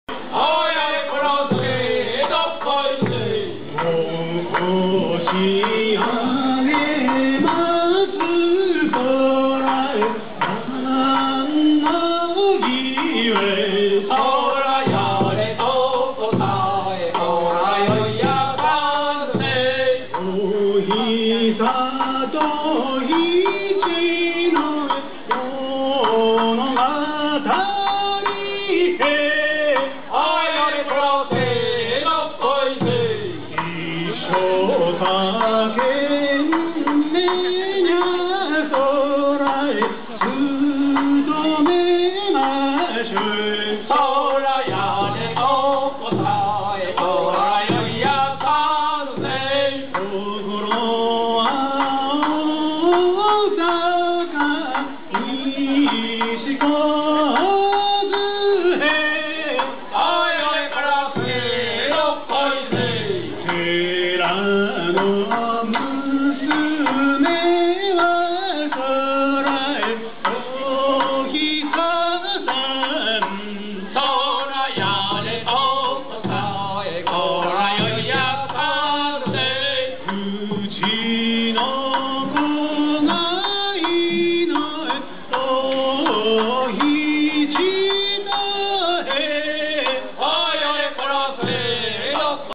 本場の正調河内音頭
2012/08/23撮影。八尾市常光寺にて。
"エ～ェ　サテワ～～ァ　いちざぁ～～～っの　みなさまうぇ～～～ええええ、ちょいと出ました　わたくしワ～っ、お見かけっ通り~~ぃの若輩で～～エエ、ヨ～こいこい"
っていう、よく耳にする河内音頭だけが、河内音頭ではないそうです。
こっちが本場の正調なんだって・・・。